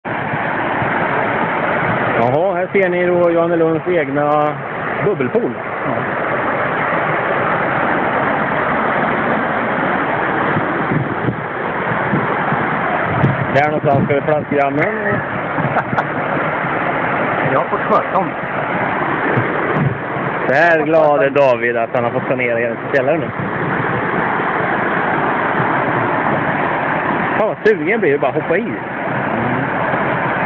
Översvämning i Johannelund / Linköping 100719
En ledning utanför plaskis har brustit och boendes fick sig en liten våt överraskning minst sagt :)